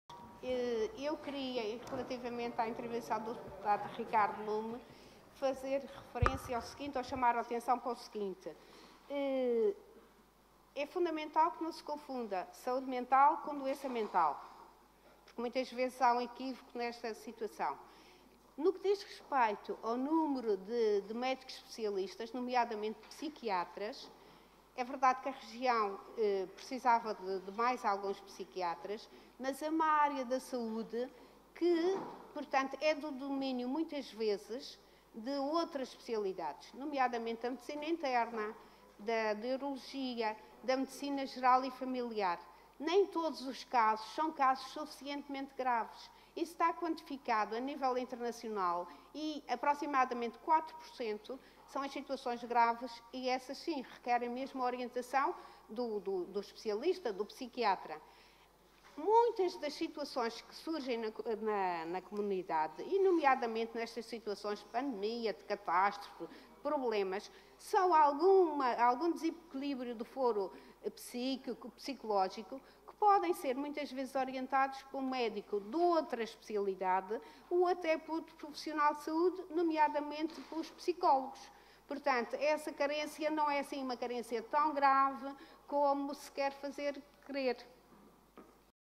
Assembleia Legislativa da Madeira | 09.06.2020
Segunda intervenção da deputada Conceição Pereira
Apreciação na generalidade do projeto de resolução, intitulado " Garantia de seguimento psiquiátrico na comunidade dos utentes internados nos serviços de psiquiatria da RAM e Implementação de um Plano Regional de Saúde Mental "